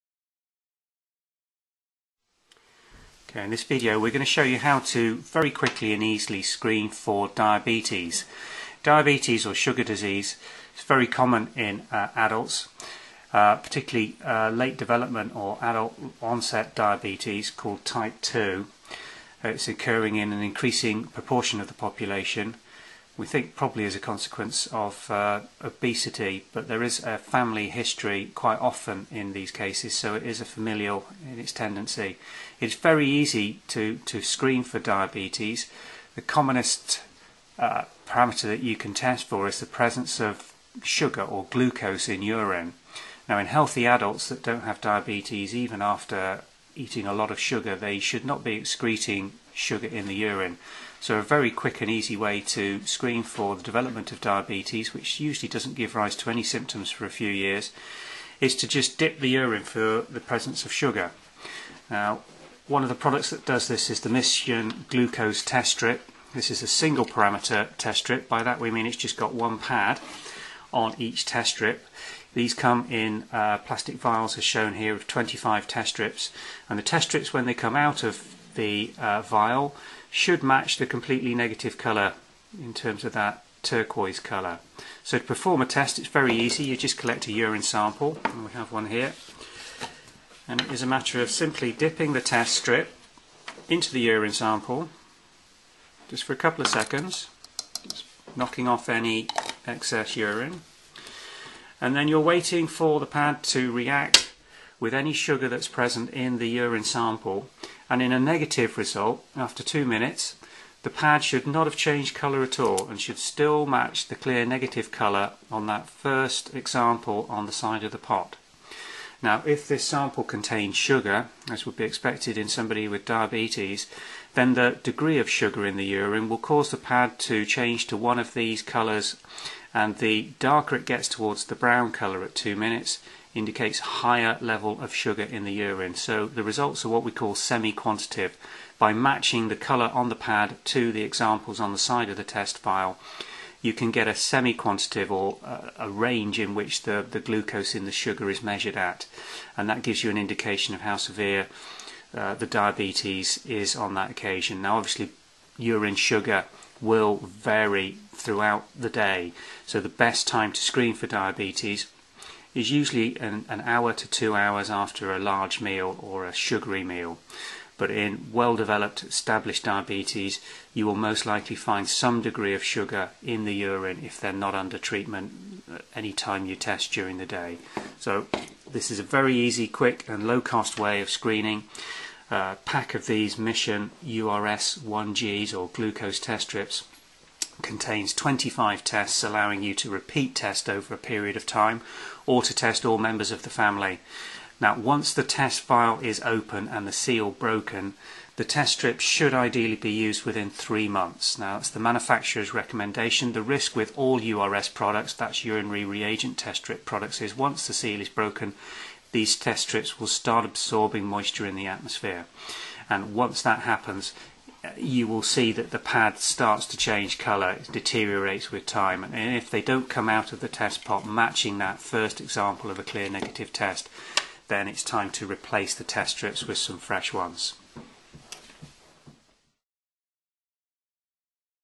How to test for Diabetes Type 2 -a quick urine test for diabetes
A really quick and easy test for diabetes using glucose test strips to test a urine sample. Can be performed at home.